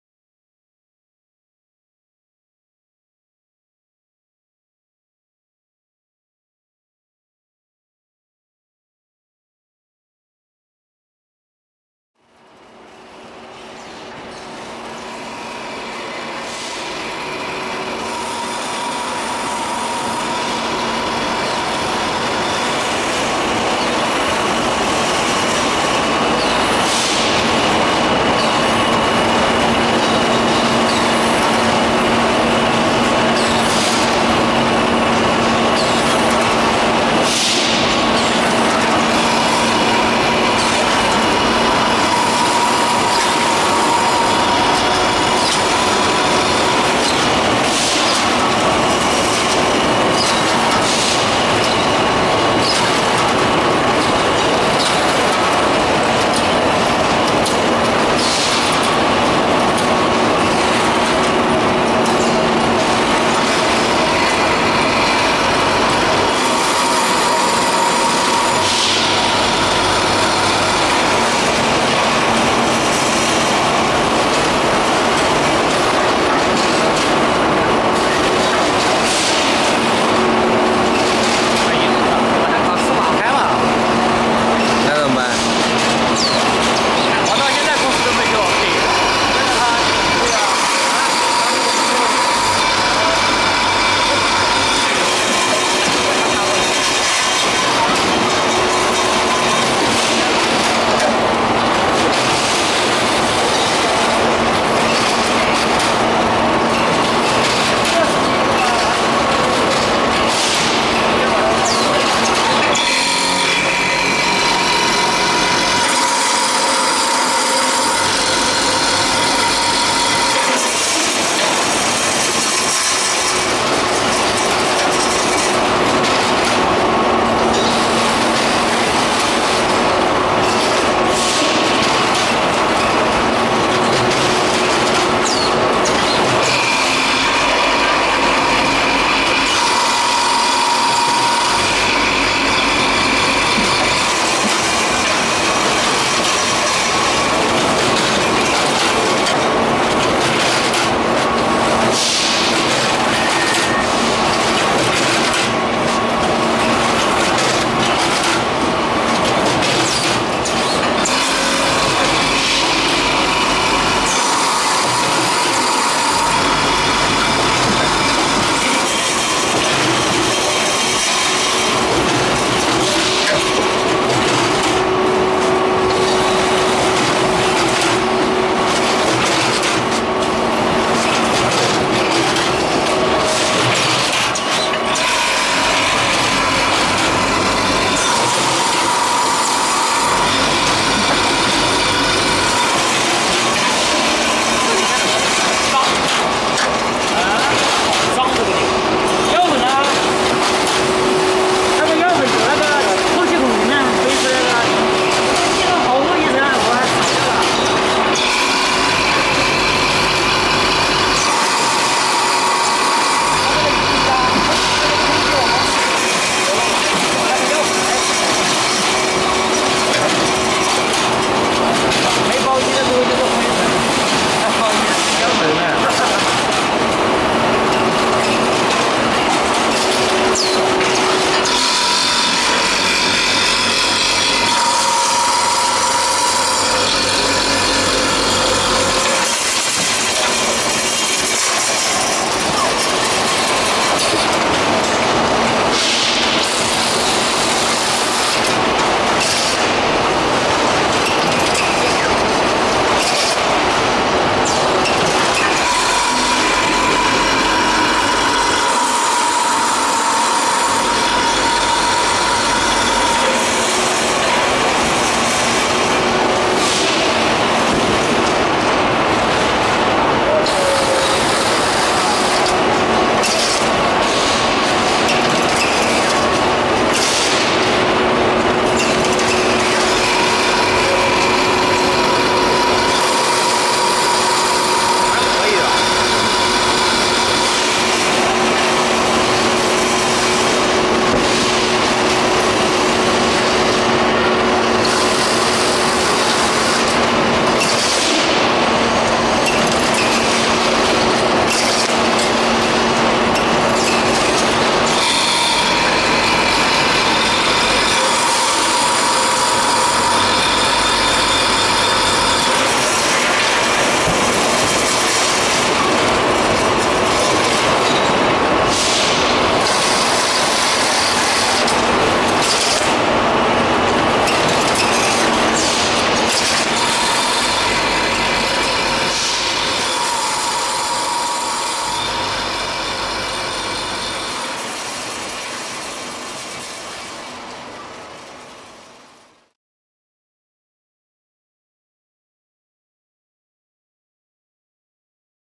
Automatic Pouch Packaging Line—AnHui ZengRan
The packing line is composed of Bulk material can be packed by packing machine，then the packed bag will be shaped,sorted,and filled in order into carton...

More About Us:
Address:No. 65 Jiqiao Rd.,Luyang Industrial Park, Fuyang Ave, Hefei, Anhui, China;